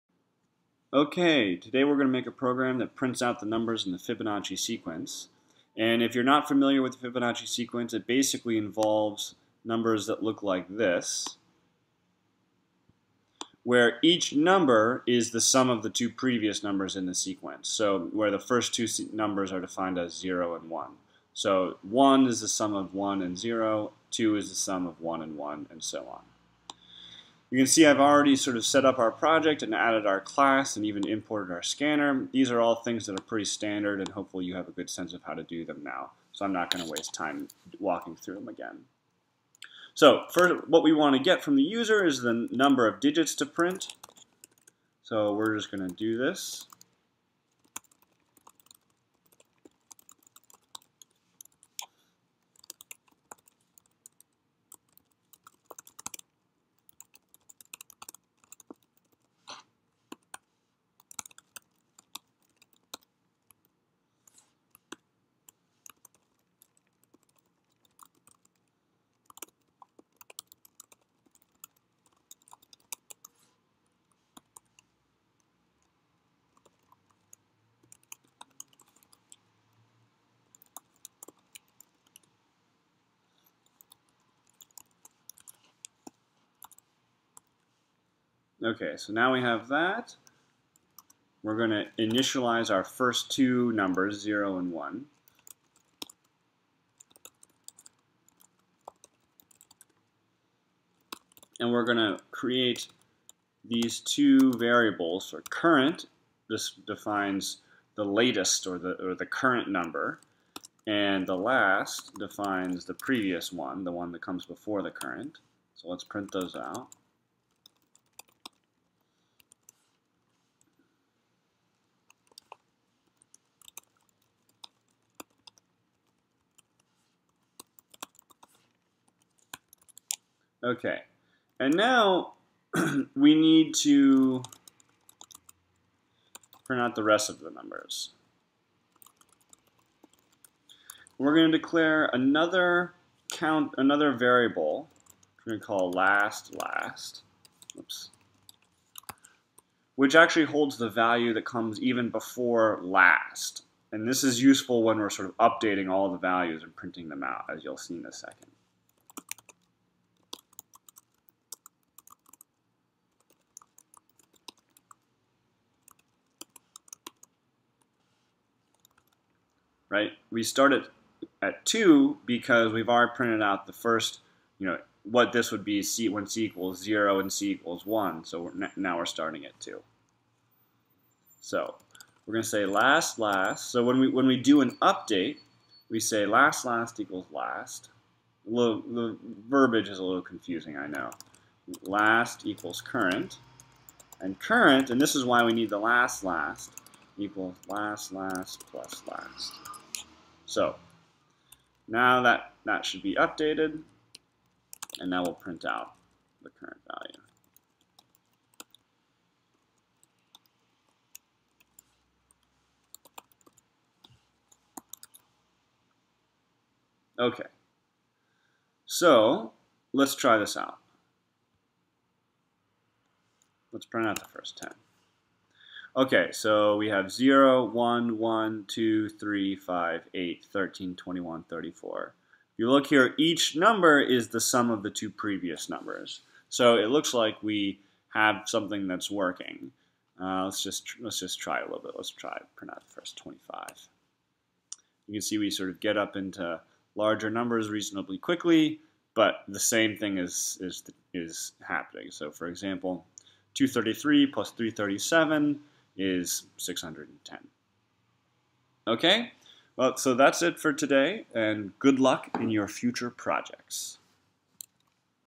Java walk through: Fibonacci
We walk through implementing a program (in Java + Eclipse) to print the numbers of the Fibonacci sequence.